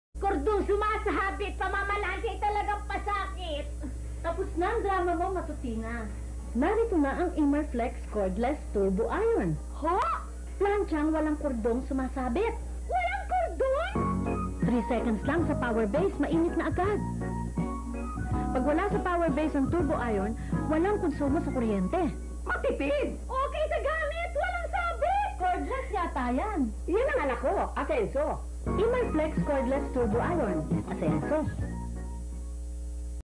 1987 Imarflex Cordless Turbo Iron TVC
A TV commercial based on the tv show John En Marsha.